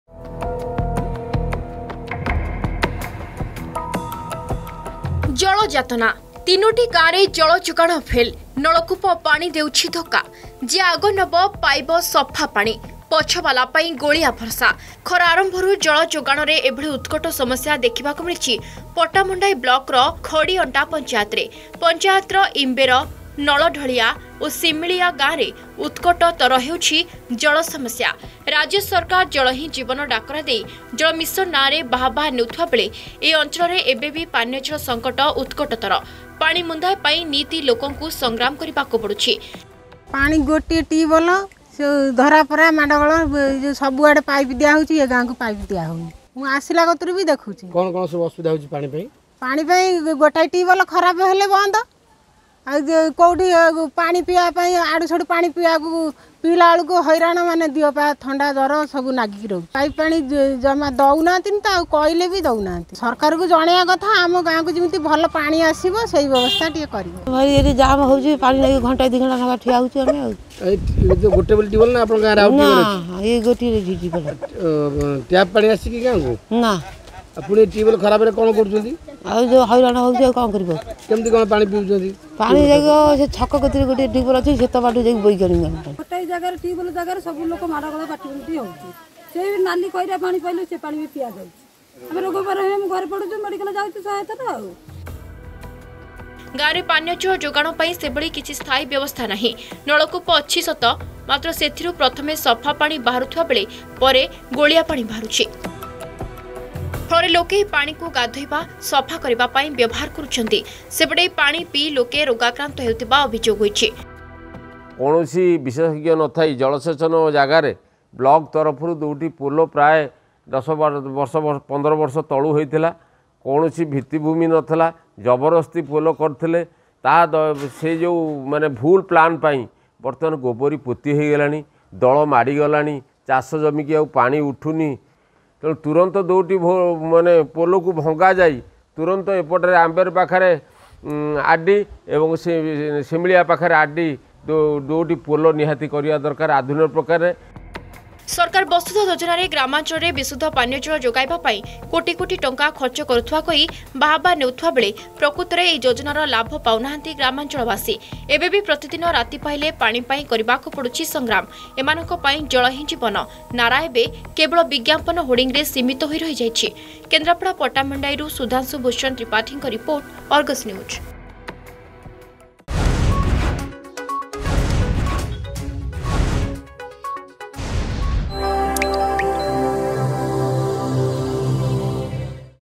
Odisha Govt's Basudha Yojana fails in Kendrapara, Villagers struggle to get drinking water
Odisha Government's Basudha Yojana fails in Pattamundai Village of Kendrapara district for which villagers struggle to get safe drinking water.